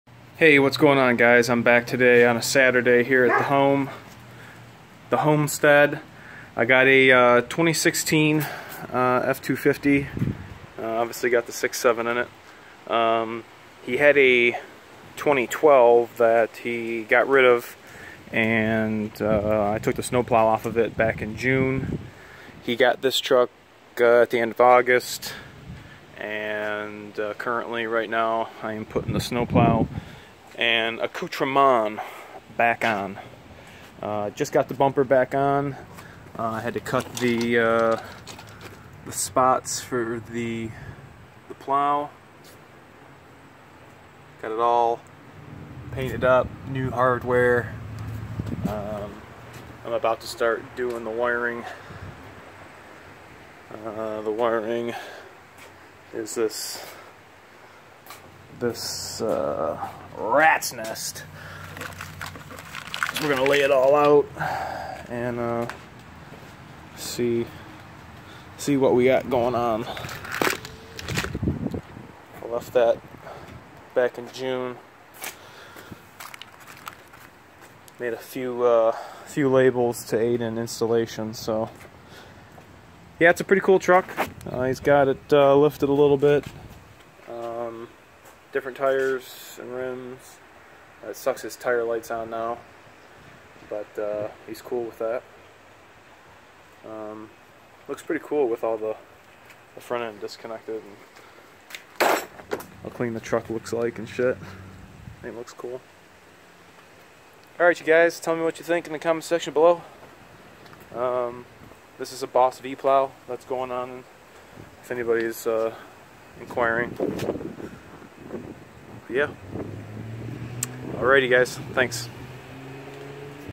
F-SUPERDUTY-BOSS PLOW INSTALL
Took about 4 4.5 hours, wasn't too bad to put back on. Worked real well.

I am the creator and owner if this material. Downloading is strictly forbidden.